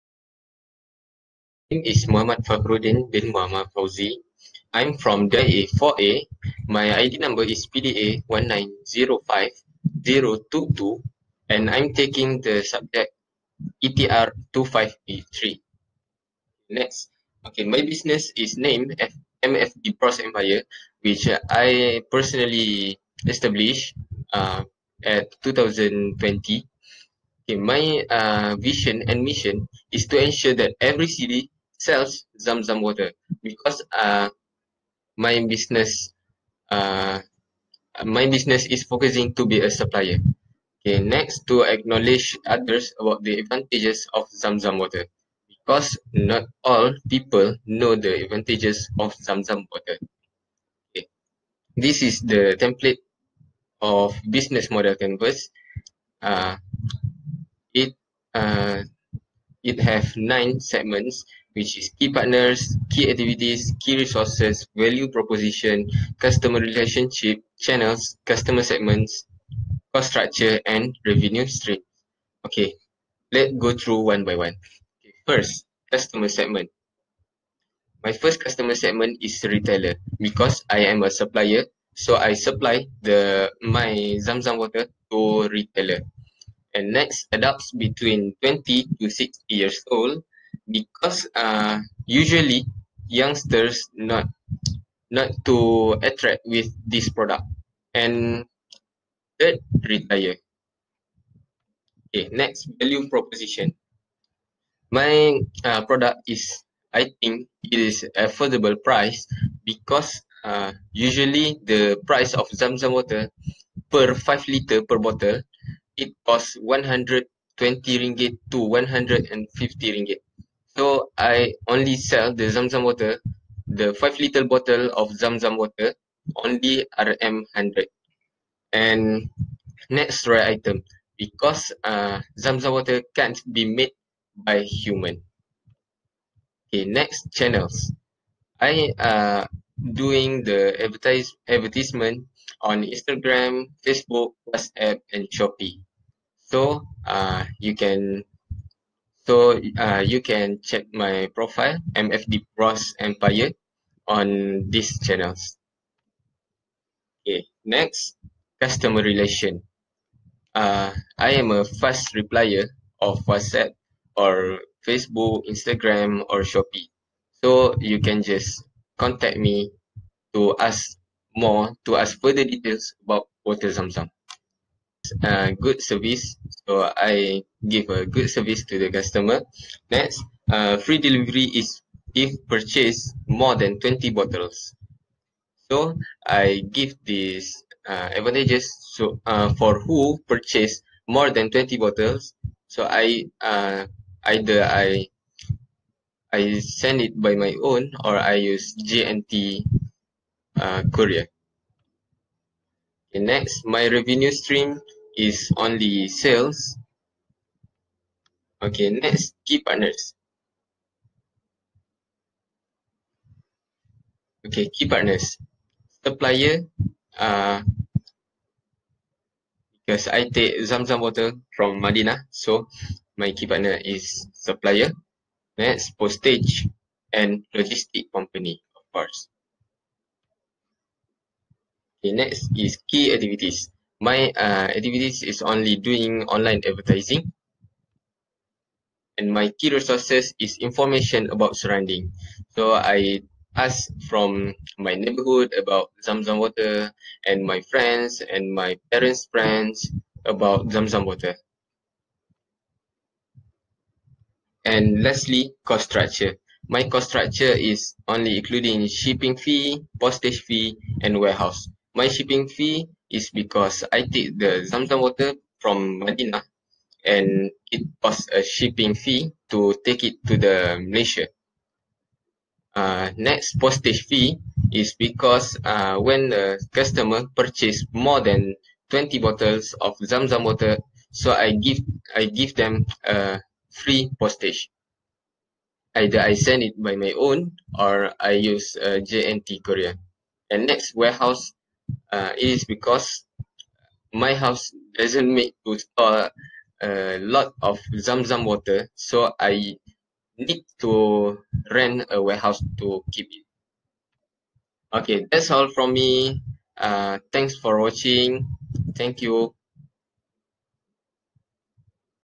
Business Model Canvas Presentation (Muhammad Fakhrudin)
Full Name : Muhammad Fahrudin Bin Mohamad Fauzi
Business Model Canvas Title : zamzam water

Hi, i am presenting a Business Model Canvas for my eUsahawan Muda class. Please COMMENT & LIKE to improve my video.